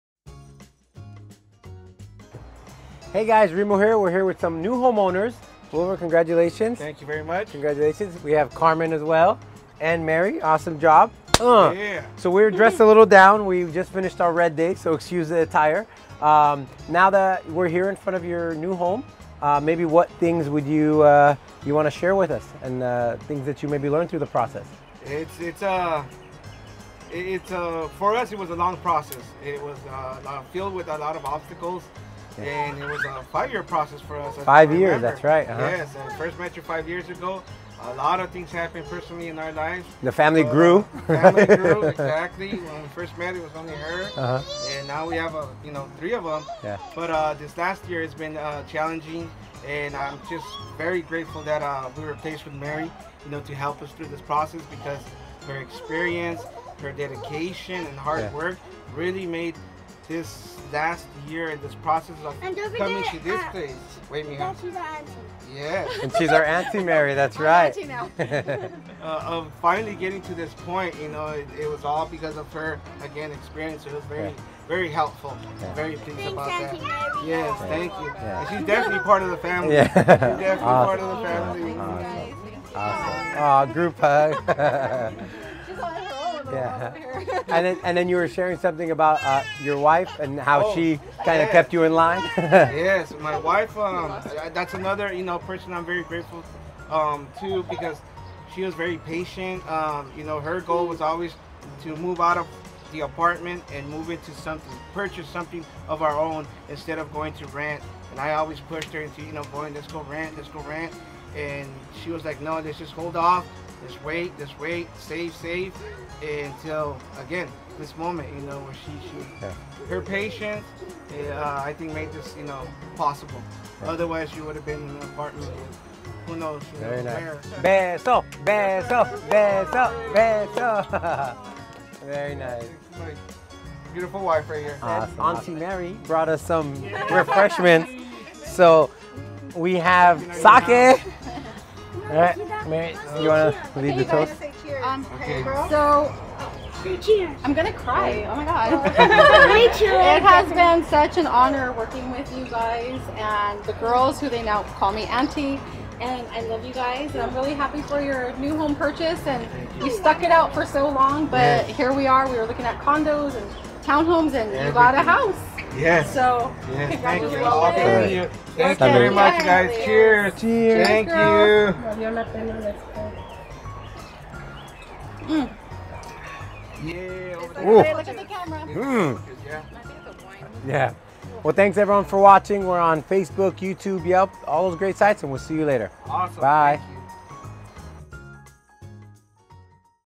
Congrats to Will and Ada for becoming Home Owners Video! with Remo the Realtor (Whittier Realtor)
Wilver and Ada came back after taking a break 5 years ago and were eager to get back into the search. They hit the floor running with our Buyers’ Specialist, Mary in September 2017, adopted her as honorary Auntie, and after viewing at least 40 homes, writing 7+ offers on potentials, the perfect place for them and their girls was the winner!

A BIG thank you to everyone who made it happen for Wilver and Ada... Carmen of New American Funding for securing their financing, the listing agent Mark Perez KW Cerritos for a smooth transaction, to Auntie Mary for doing a spectacular job on finding them their beautiful new home, and especially to Wilver and Ada for allowing us to be part of this milestone for you and your girls!

We are wishing you all the best! Enjoy the summer with Family and Friends, we will be over for a few BBQ’s! Cheers to the new adventures and it was a pleasure representing you in the purchase of your beautiful new home.

Thinking about Buying in 2018 with Remo the Realtor? We are pretty good at that stuff